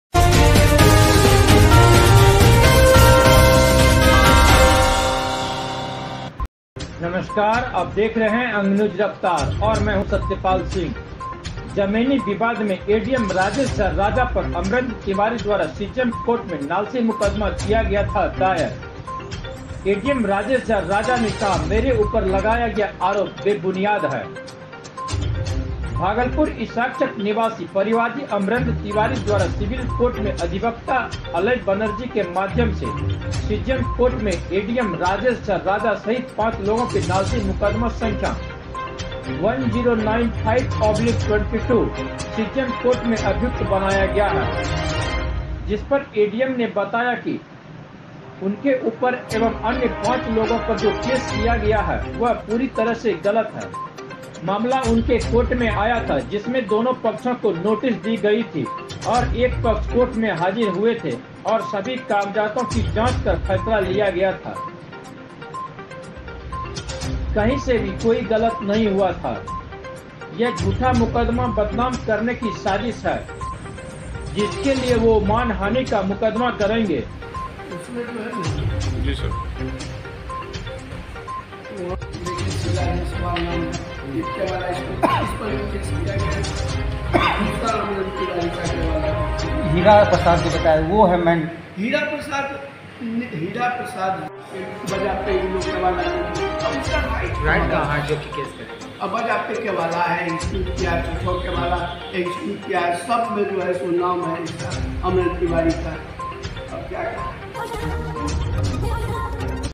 एडीएम पर नालसी मुकदमा किया गया था दायर,एडीएम ने कहा "मेरे ऊपर लगाया गया आरोप बेबुनियाद है"।
*जमीनी विवाद में एडीएम राजेश झा राजा पर अमरेंद्र तिवारी द्वारा सीजीएम  कोर्ट में नालसी मुकदमा किया गया था दायर , एडीएम राजा राजा ने कहा मेरे ऊपर लगाया गया आरोप बेबुनियाद है*

एंकर -भागलपुर  इशाकचक निवासी परिवादी अमरेन्द्र तिवारी द्वारा सिविल कोर्ट के अधिवक्ता आलय बनर्जी के माध्यम से सीजीएम कोर्ट में एडीएम राजेश झा सहित पांच लोगों को नालसी मुकदमा संख्या 1095/22 सीजीएम कोर्ट में अभियुक्त बनाया गया है।जिसपर एडीएम ने बताया कि उनके ऊपर और अन्य पांच लोगों पर जो केस किया गया है वह पूरी तरीके से गलत है। मामला उनके कोर्ट में आया था जिसमें दोनों पक्षों को नोटिस दी गई थी और एक पक्ष कोर्ट में हाजिर हुए थे और सभी कागजातों की जांच कर फैसला दिया गया था। कहीं से भी कोई गलत नहीं हुआ है। यह झूठा मुकदमा है बदनाम करने की साजिश है। जिसके लिए वह मानहानि का मुकदमा दर्ज करेंगे।
BYTE-
राजेश झा राजा, एडीएम भागलपुर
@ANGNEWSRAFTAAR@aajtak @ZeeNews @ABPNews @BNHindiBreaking